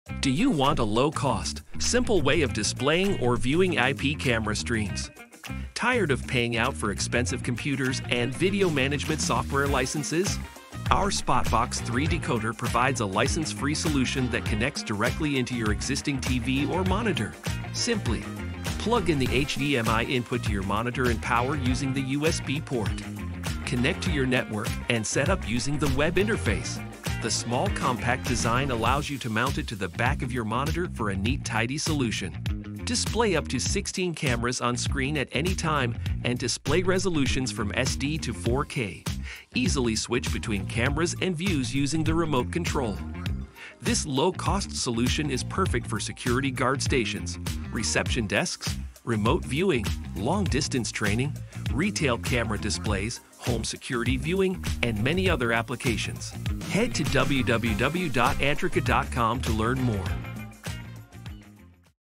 Spotbox Decoder : A simple multi view IP camera and ONVIF Decoder
The Spotbox is a simple 1-16 camera or RTSP ONVIF stream decoder with remote control